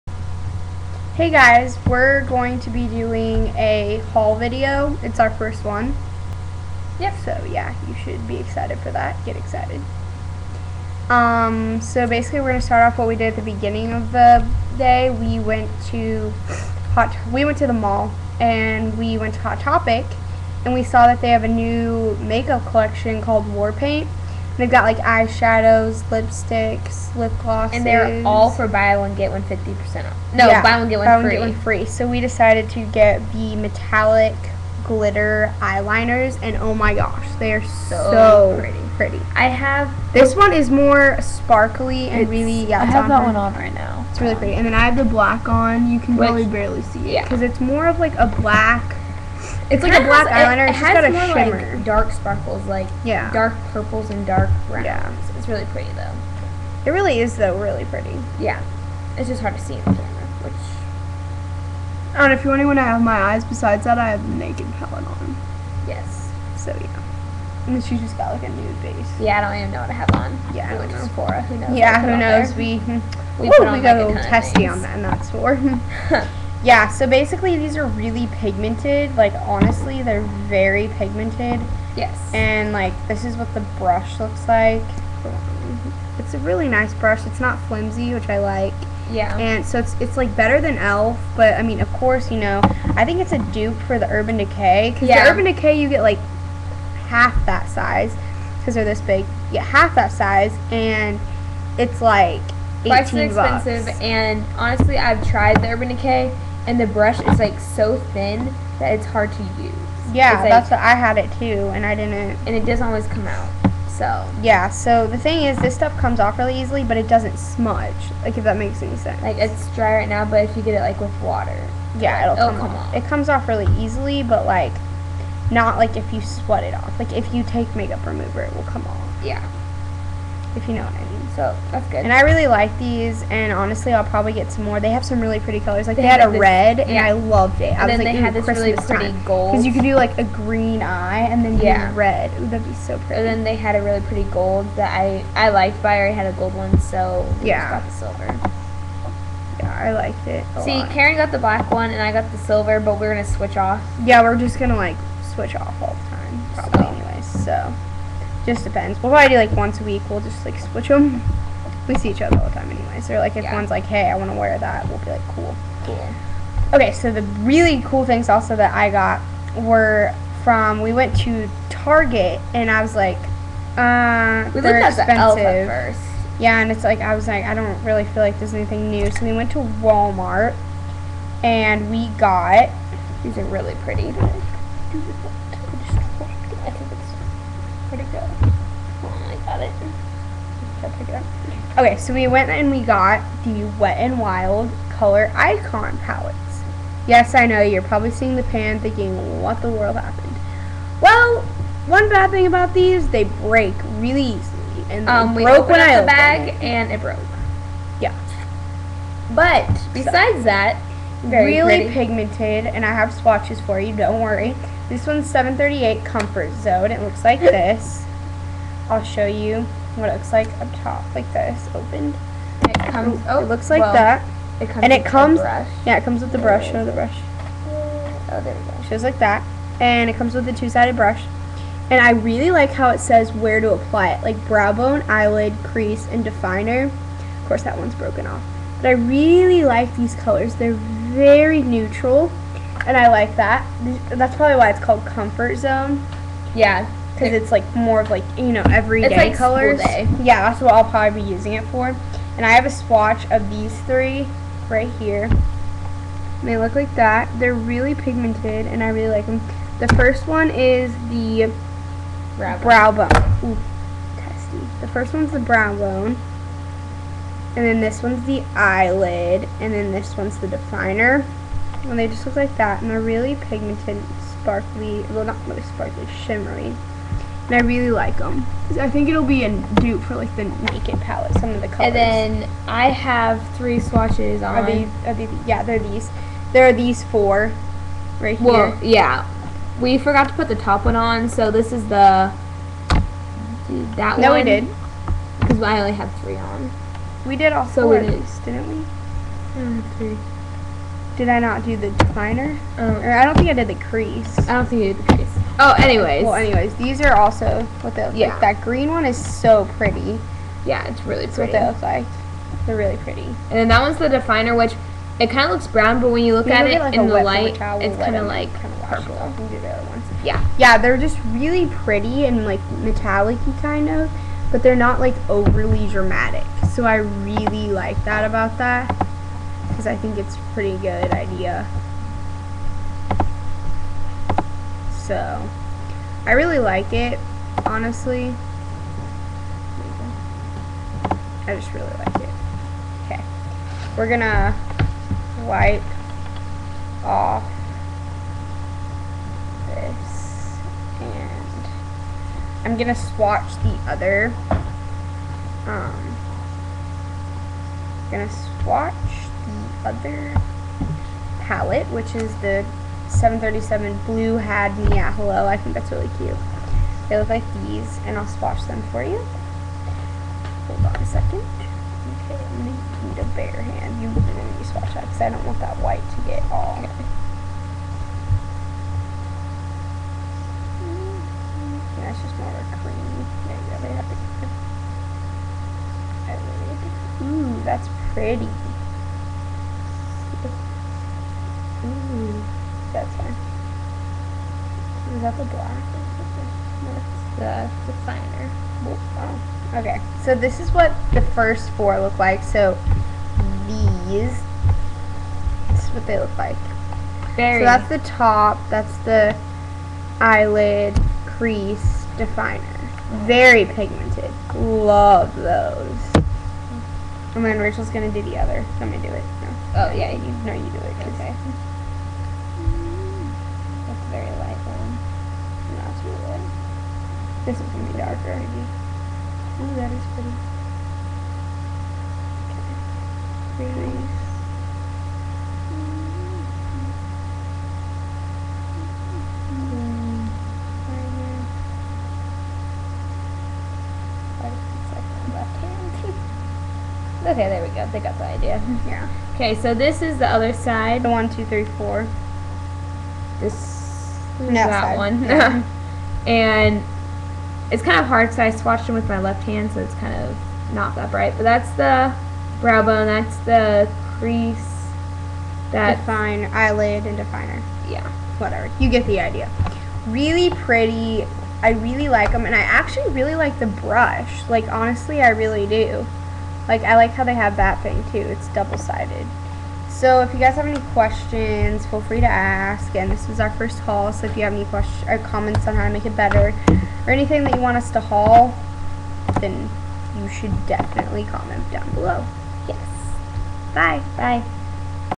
first makeup haul!!
haulin time!!! :) wet and wild eyeshadow palettes in comfort zone and blue had me at hello - walmart-5 dollars , warpaint glitter eye liner in silver and black sparkle - hot topic- 5 bucks- buy one get one free!! :)